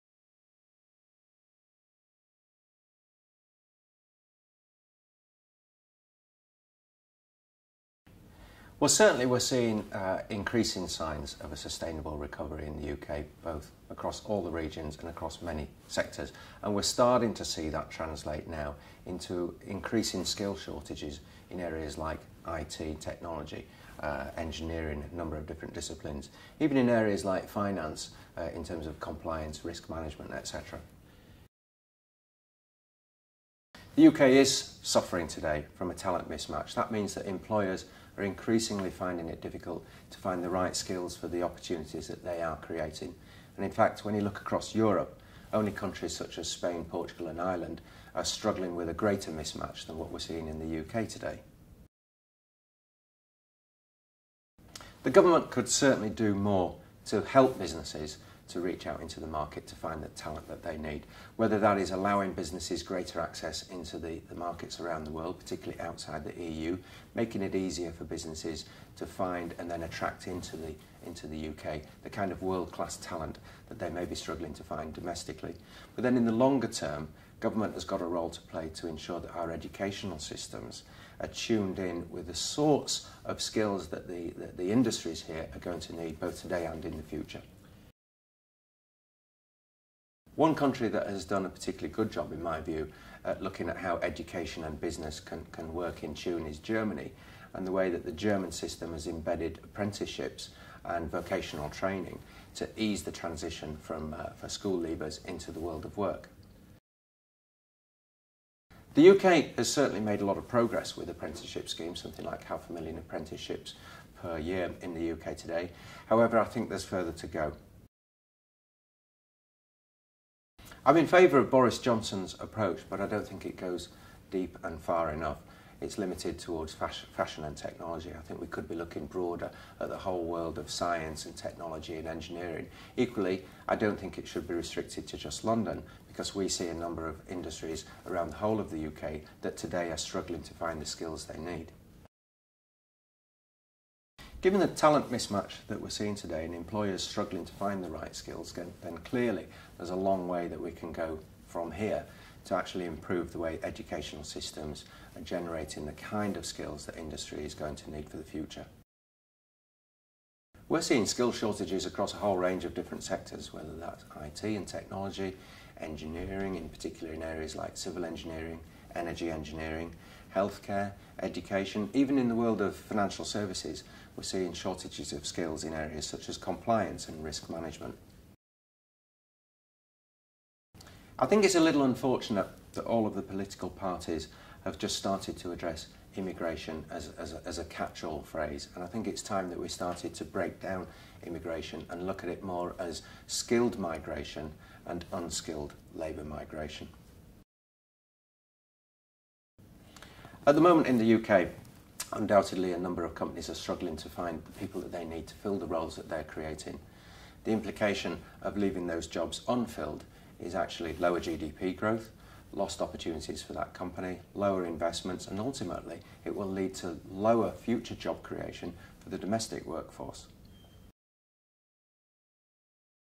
Hays Recruitment
A corporate film featuring interviews from the CEO following an announcement